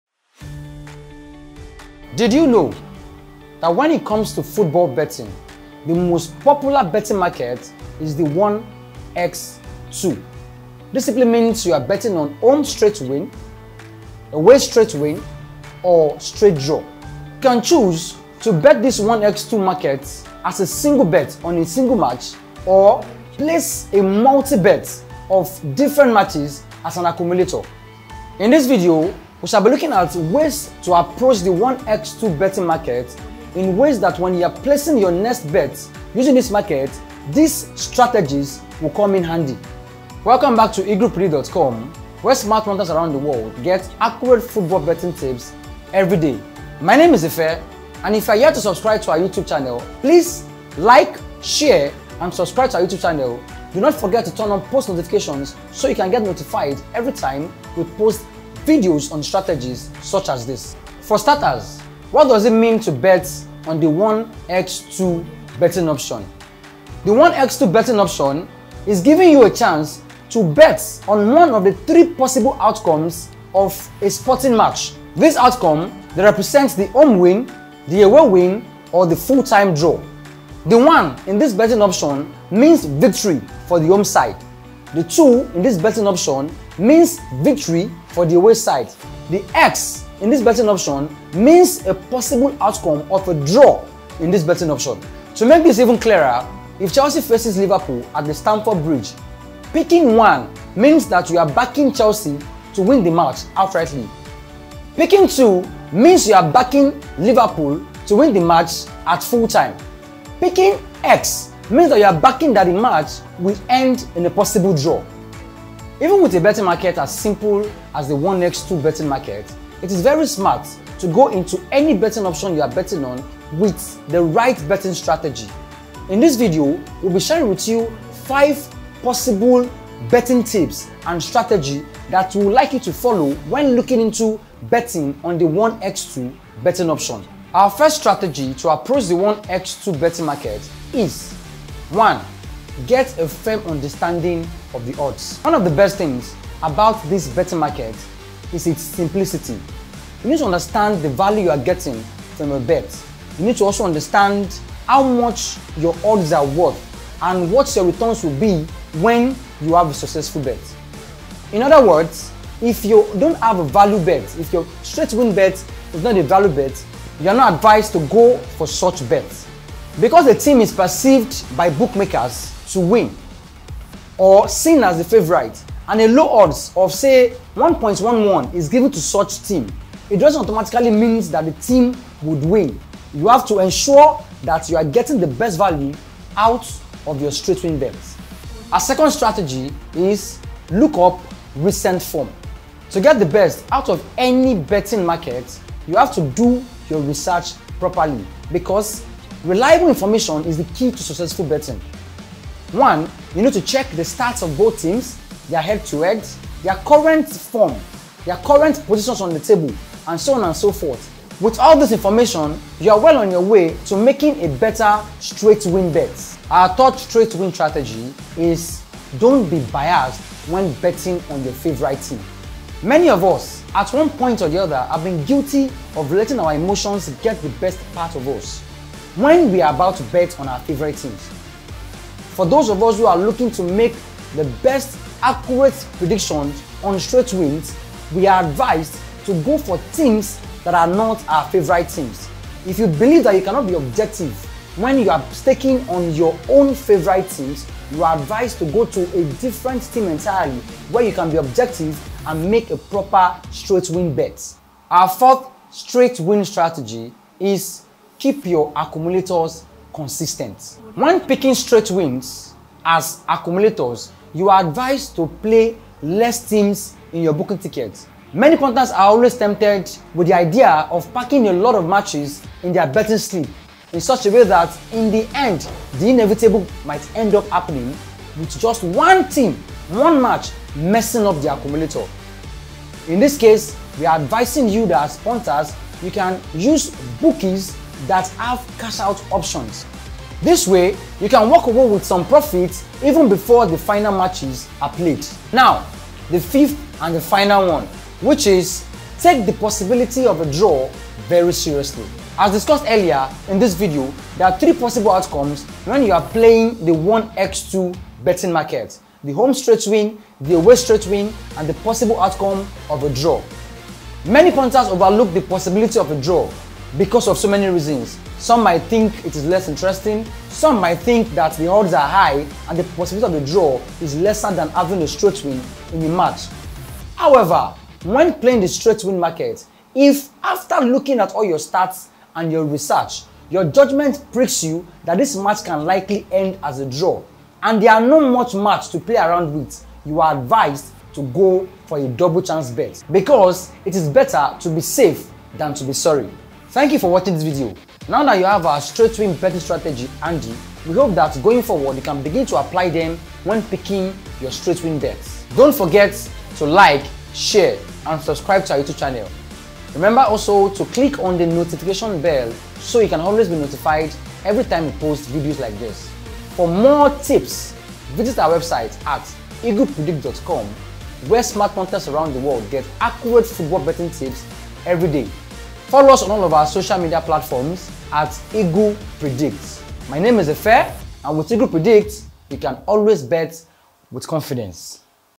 Straight Win Betting Strategy - 5 Ultimate Secrets to increase your chances of Winning your Bets
When it comes to football betting, the most popular betting market is the straight win betting strategy, this is simply a bet on either a home win, away win or the draw.

to get started

You can choose to bet the 1x2 market on just one match and therefore place a single on a particular team or alternatively you can place a 1x2 multiple bet that features two or more selections.

In this Video, we look at the 5 ultimate secrets to increase your chances of winning using our Straight win betting strategy which could come in handy should you decide to test the market on your next set of bets.

What Does Straight win Means?

1X2 betting has you bet on three possible outcomes on a sporting event.

These outcomes include a home win, an away win, and a draw.

The 1 represents the home team victory, the 2 represents the away team victory, and the X represents the possible draw.

To make it even clearer, if Chelsea face Liverpool, then the 1 would represent Chelsea winning, the 2 Liverpool winning and the X as a draw.

The reason it is written this way is because that's how it often used to appear on physical betting slips from betting shops and numerous online betting websites

Even with a betting market as simple as straight win betting, it's smart to go into it with a solid strategy in mind. As is always the case with sports betting markets, there's a lot of research and thought that goes into a smart bet.

With that in mind, we've decided to whittle it down to five of our straight win best tips that we think you'll want to follow to get the best out of your straight win bets.

1. Get a firm understanding of the odds
One of the finest aspects of this market is its simplicity, which makes determining the worth of your bet a lot easier. You'll need to calculate the value of your odds. In other words, the amount of money you'll collect if your wager pays off. It is not wise to place a value bet if your wager is not a value bet. Also, just though a team is regarded by bookmakers to win and has been assigned extremely low odds, such as 1.11, does not guarantee that the team will finally win.

2. Look up recent form
Remember to do your homework since accurate information is essential for winning bets. Before betting on a football match, look at the league table, both teams' current form, the latest news, and their head-to-head records.

3. Don't just bet on who you want to win
Although this may seem self-evident, I believe we have all been guilty of allowing our emotions to get the best of us when it comes to our favorite teams. If you want to produce the most accurate straight win betting predictions, you'll need to consider whether your forecast is based on what you believe will happen or what you want to happen. If you don't think you'll be able to remain objective, choose a game where you can.

4. Keep your accumulators consistent
When placing a multiple bet, choose a smaller number of teams. It's easy for a punter to be tempted to fill up an acca with too many favored teams in the hopes of making money. However, it is important to remember that at least one team will inevitably make a mistake. On accumulator bets, look for bookies that provide Cash Out choices. It means walking away with profits without having to wait for the final matches to be played.

5. Take the possibility of a draw seriously
This is a really easy one, but we thought it was important to highlight. As we've mentioned, there are three conceivable outcomes in this market. The draw, on the other hand, is frequently neglected since it is perceived as the less intriguing outcome. If a prospect of a draw pricks your judgement after you have carefully examined all research and information for a particular match, go for a double chance bet. It's always preferable to be safe than sorry.

Conclusion
When it comes to betting markets, the simplicity of the Straight win betting strategy has made it an effective and simple technique to utilize for both novice and experienced punters.

The Straight win betting method is a basic technique to betting chances that even inexperienced players may understand. Accumulator and Double chance bets allow you to increase your winnings while lowering your stake. Above all, the straight betting strategy, among others, will provide you with frequent winnings since it has the best possibility of winning.

Needless to say, although being most popular in football, straight win betting continues to catch the attention of bookmakers in other sports.

Twitter.com/eaglepredict